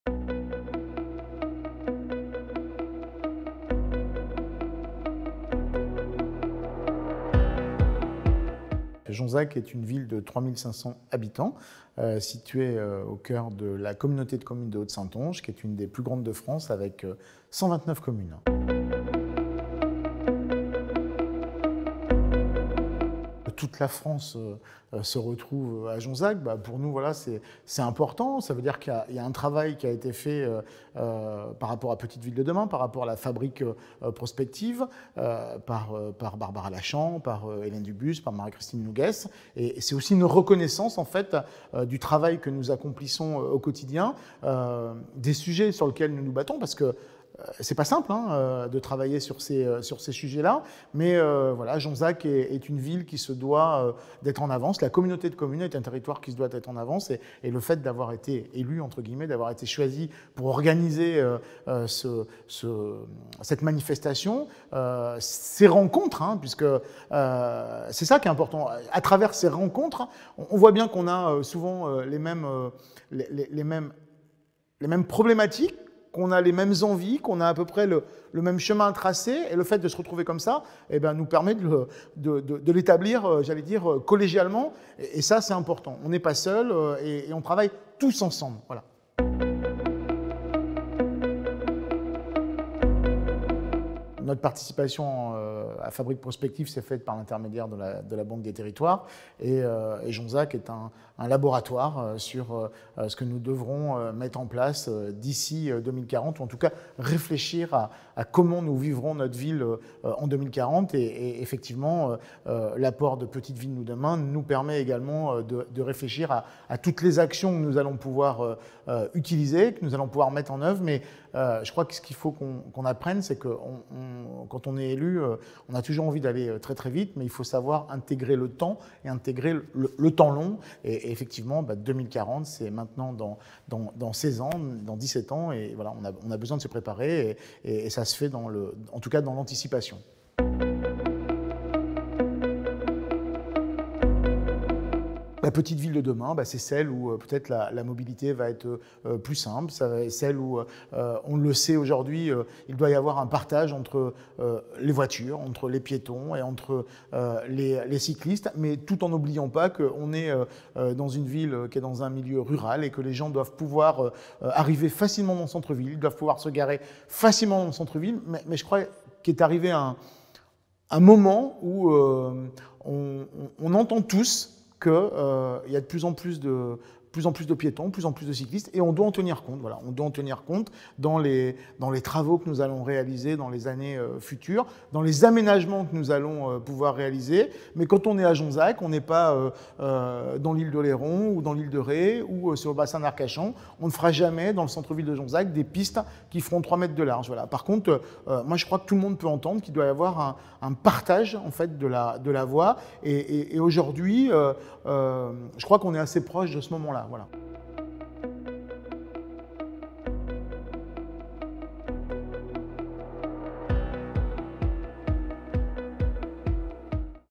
L'interview minute | Christophe Cabri, maire de Jonzac (17)
L’Agence nationale de la cohésion des territoires (ANCT) a organisé le second Atelier in situ, les 13 et 14 juin derniers, en Charente-Maritime, en partenariat avec l’association des Petites Villes de France et Popsu-Territoires. Le programme Petites Villes de demain a convié 60 élus, chefs de projet et techniciens. En question, les enjeux de transition démographique des territoires. Les cas de deux communes – Jonzac et Montendre, situées dans la communauté de communes de La Haute Saintonge – ont illustré ces thématiques.

Vidéaste : Wilfried Duval